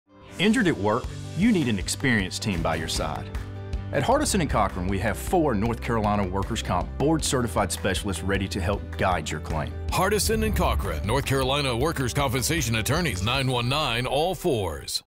The Experienced NC Workers Compensation Team at Hardison & Cochran | NC Work Injury Lawyers
Hardison & Cochran is a North Carolina Personal Injury Law Firm that handles North Carolina Workers' Compensation claims. If you have been injured at work and are looking for an experienced team to stand by your side, we have 4 North Carolina Board-Certified Workers' Compensation Attorneys ready to assist you.